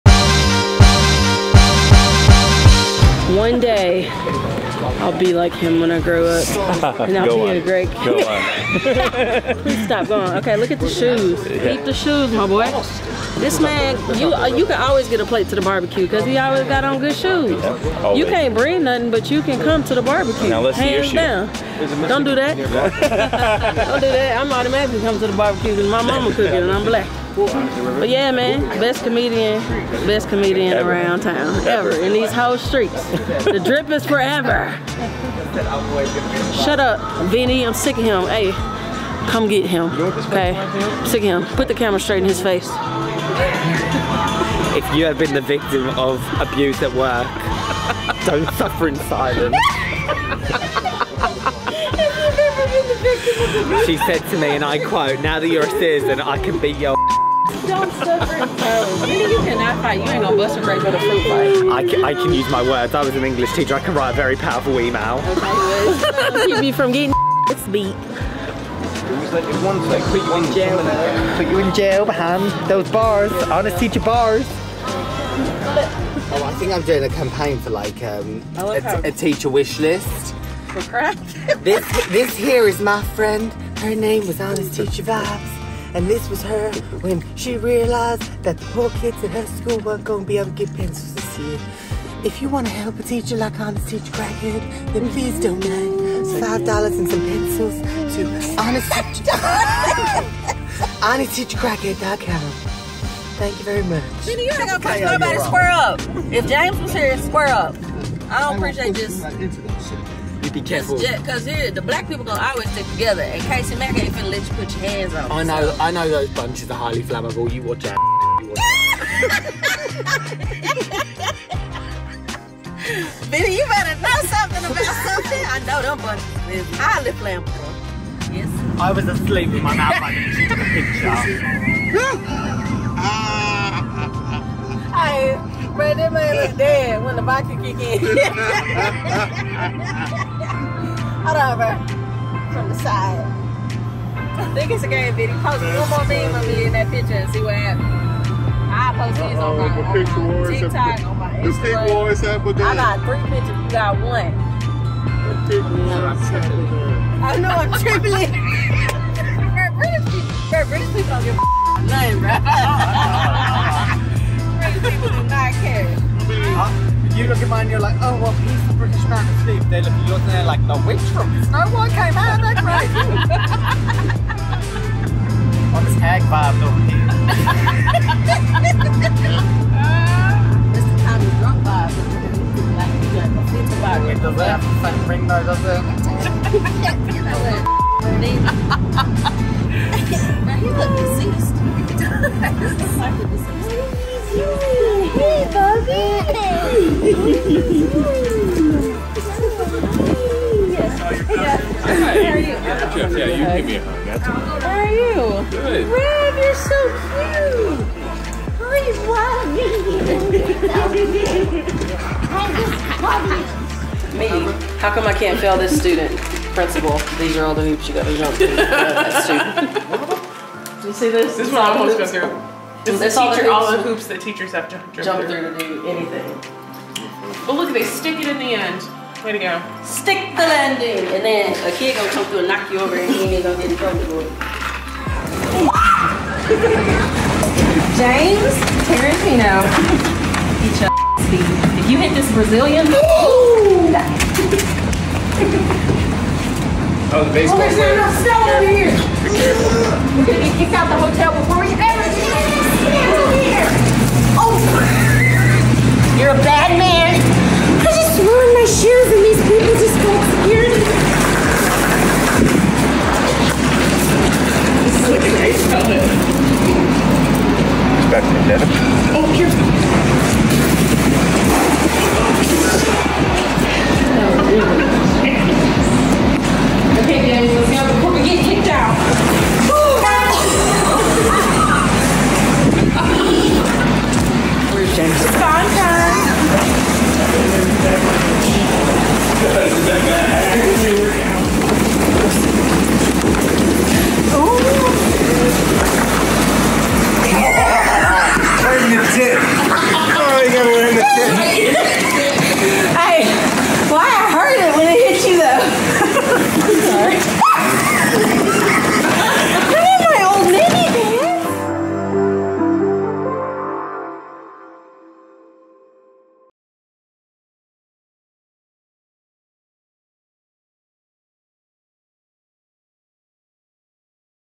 Behind the Scenes: BT Comedy Tour in the Northeast (Part 2)
The Bored Teachers Comedy Tour Vlog on our way from Philly to Boston, Episode 4!